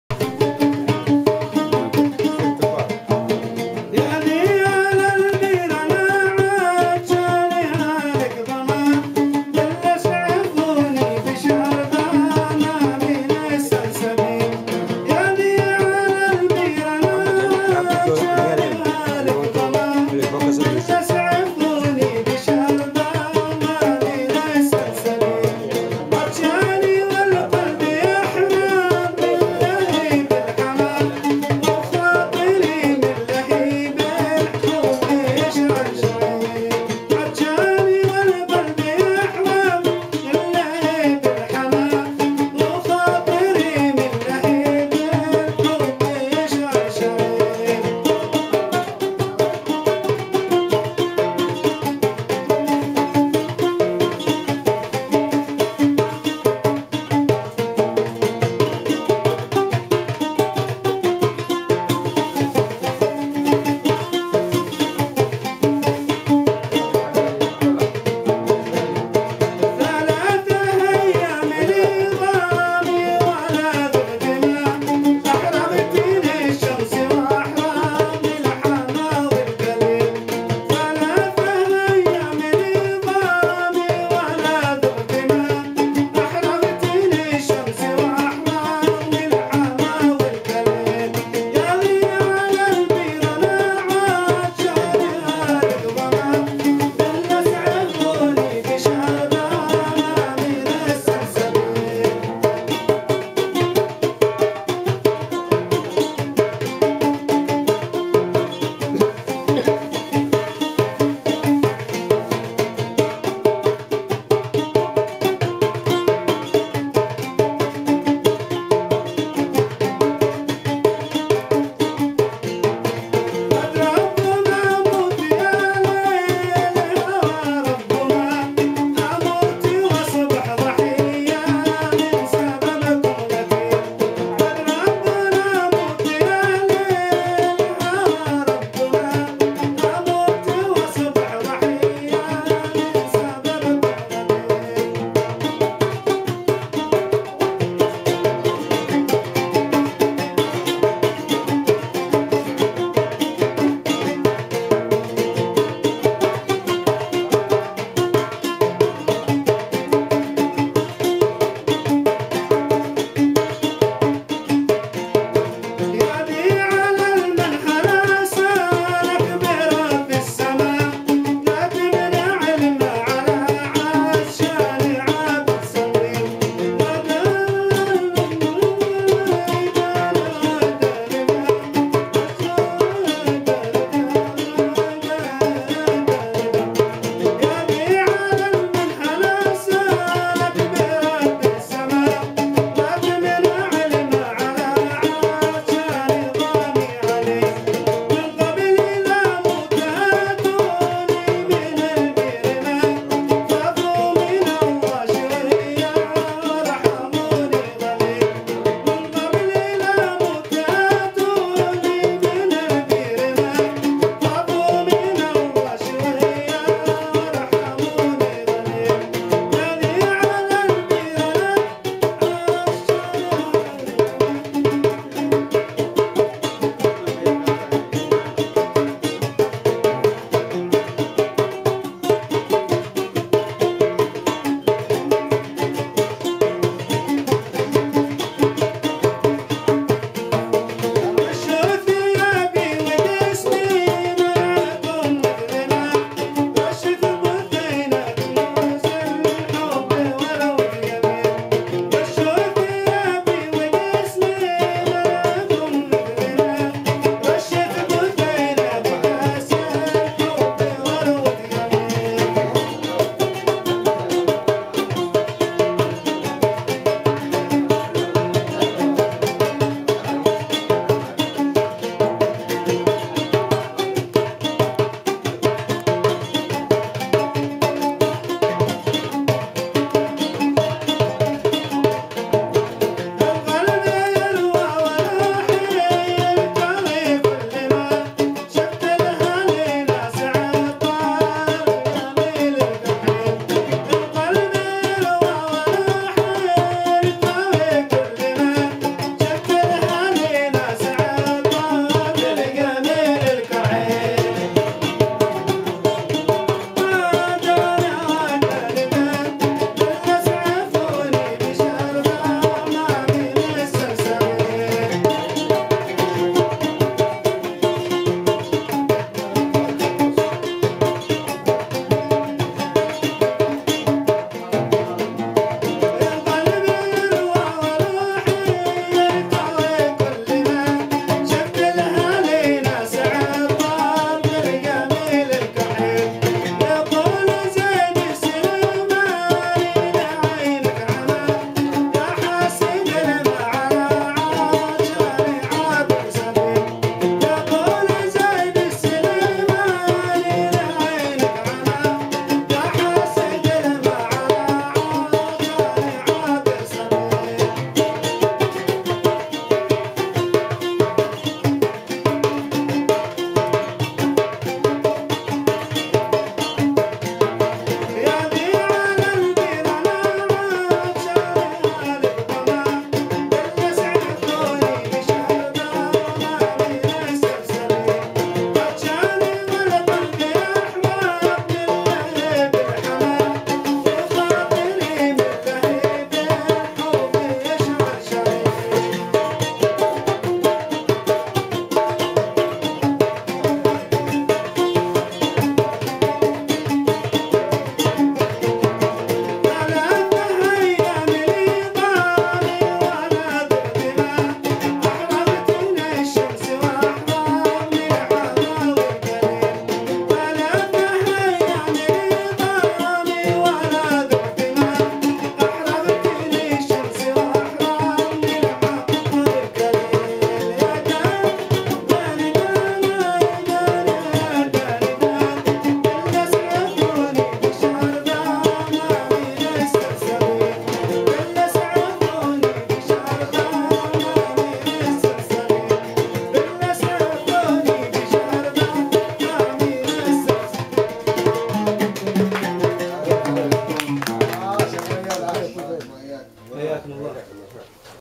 Yaffa 2009 HD Music - Ud/Vocals/Drums
Yaffai music. Old poetry performed by Ali (vocals/ud) and Hashem (drums).